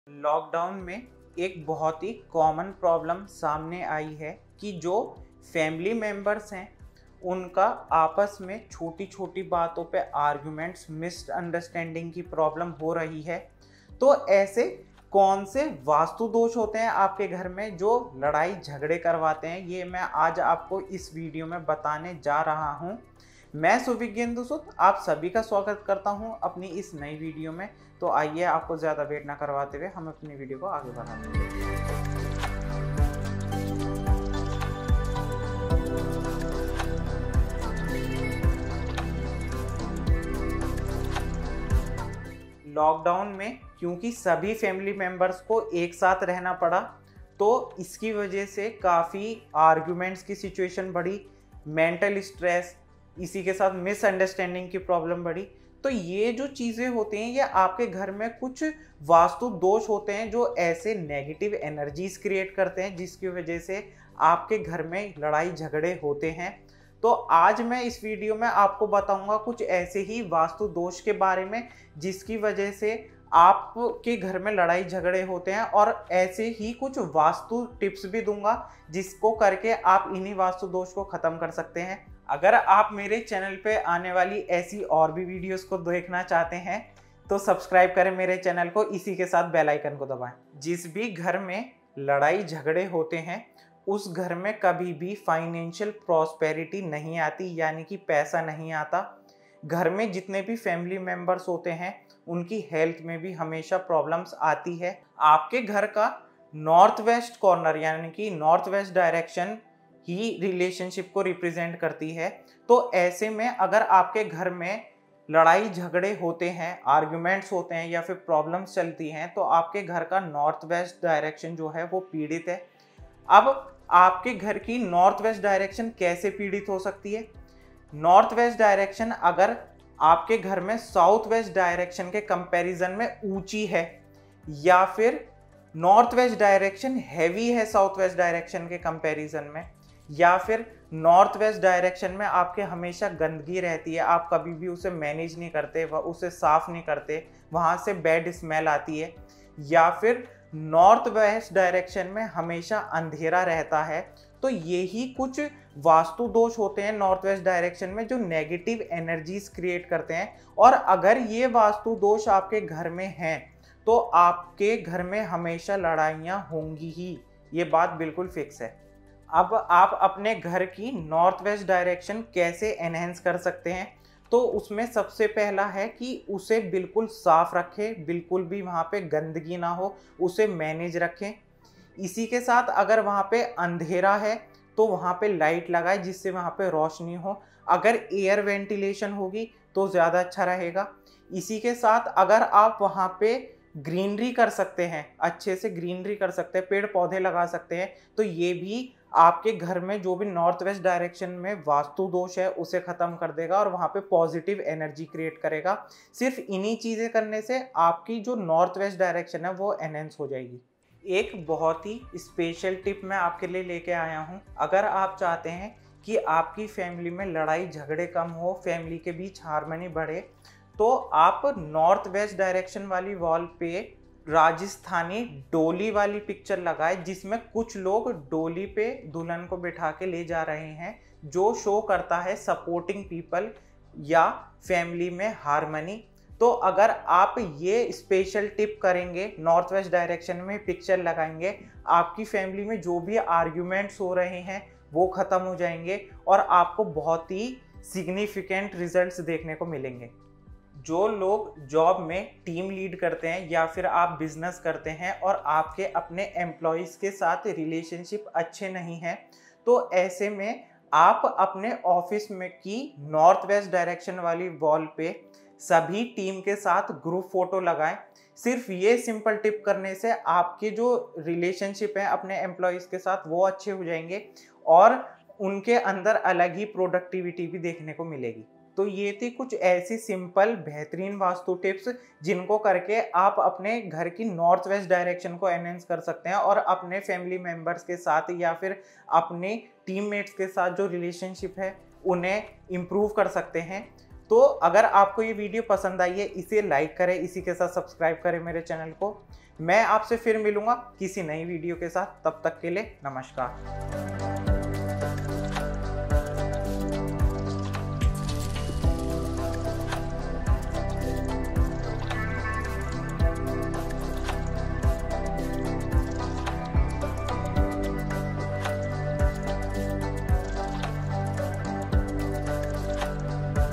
Vastu Tips To Remove Clash Between Family Members||Suvigya Indusoot
Vastu tips 2021

I’ve been getting the most common questions from the past couple of months that there is a lot of conflicts between family members after 1st lockdown so today I am explaining all the vastu dosh behind fights between family members and how to remove them.

ll Shri Hanumate Namaha ll